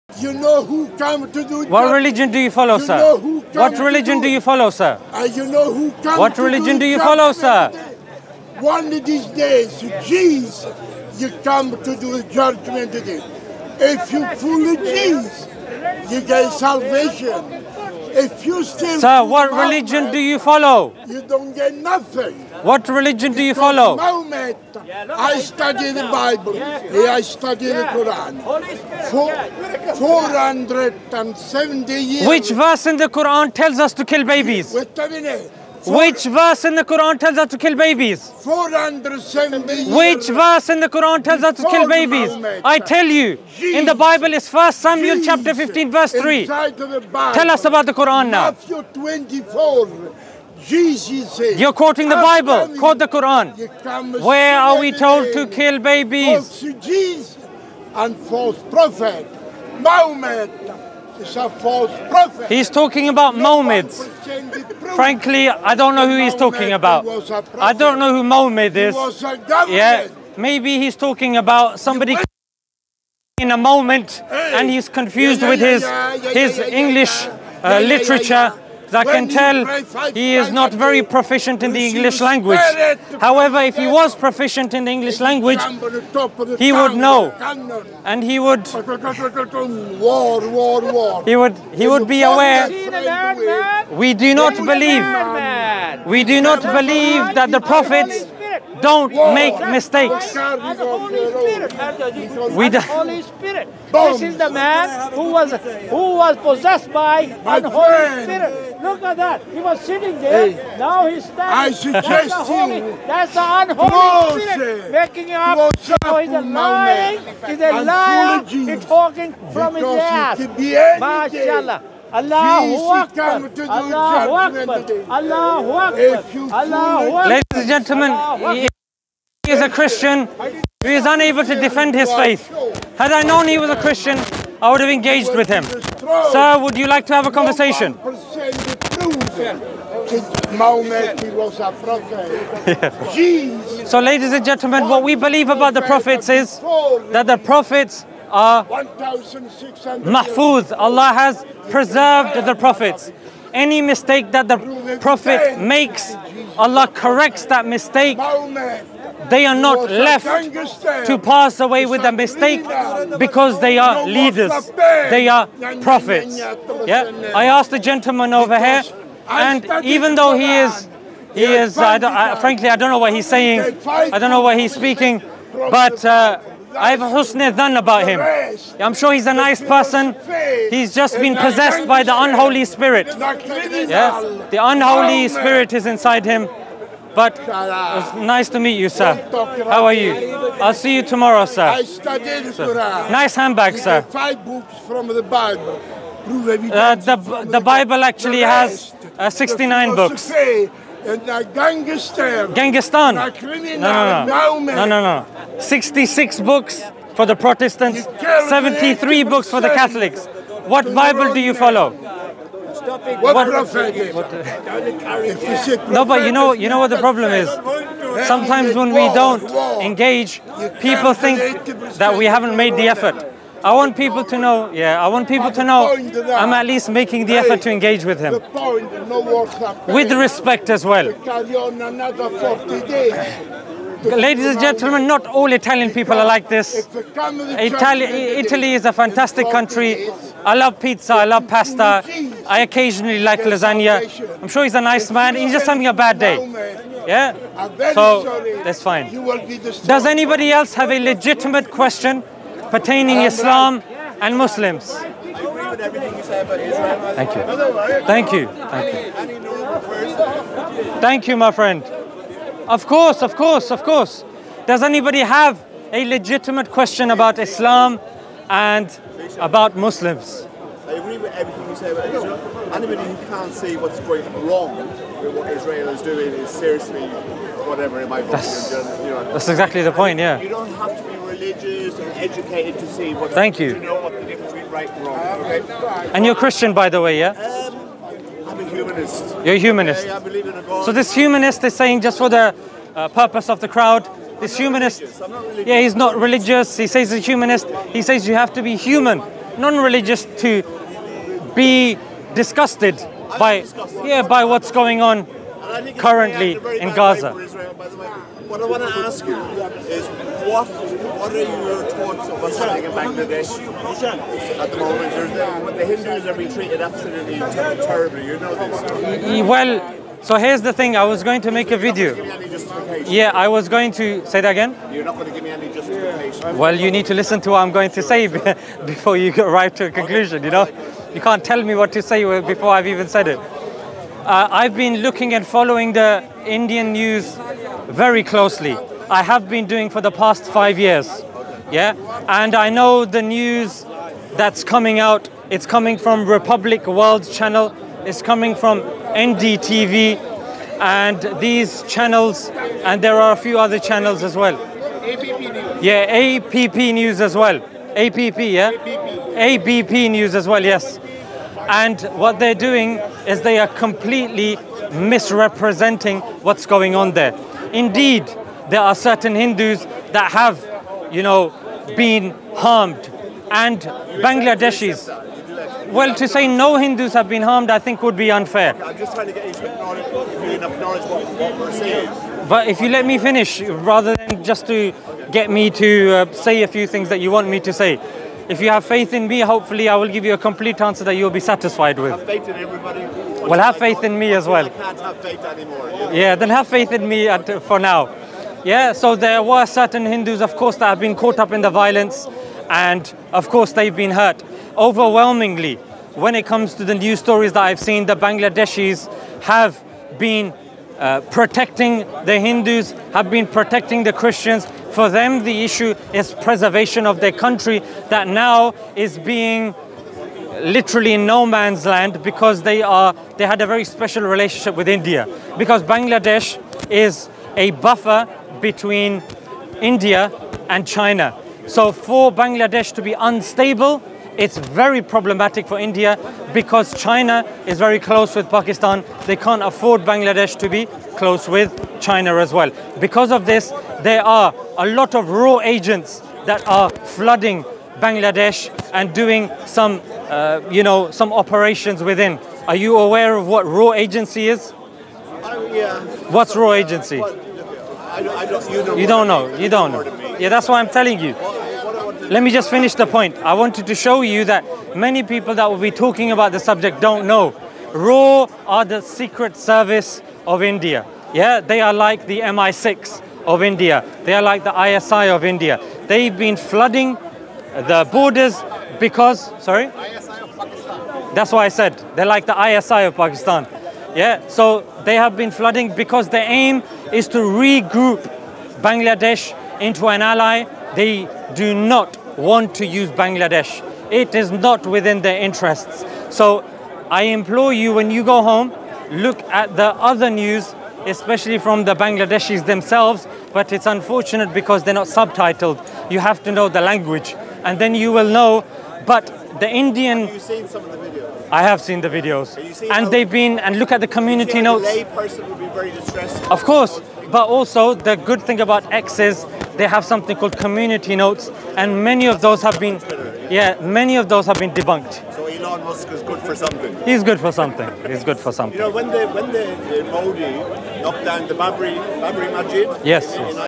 ZIONIST CONFRONTS SMILE2JANNAH, & ENDED UP CRYING! SPEAKER'S CORNER
May allah Reward You.
Some Videos May Contain Strong Words, Some Viewers May Find Some Of The Contents Offensive Or Inappropriate.
Original contents :  @sFDawah
Copy Right Reserved :  @sFDawah
Edited and Uploaded:  @sFDawah